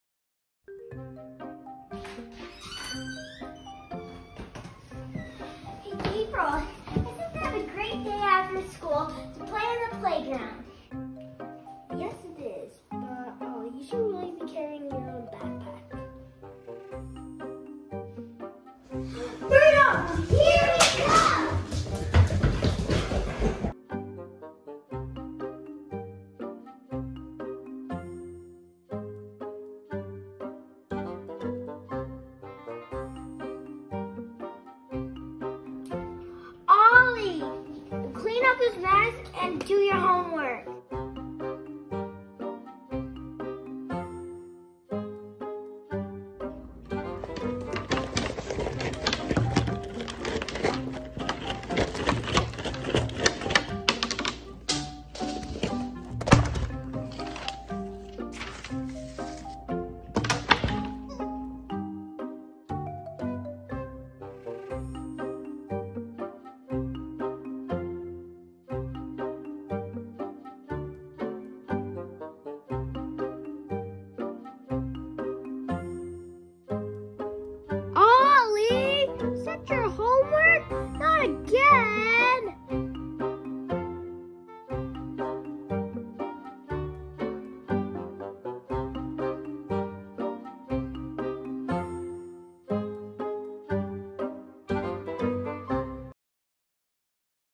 Ollie has homework?
ollie + homework = ❌ Chapters:
0:00 we’re home!
1:07 Ollie does his hamwork
1:15 OLLIE!!!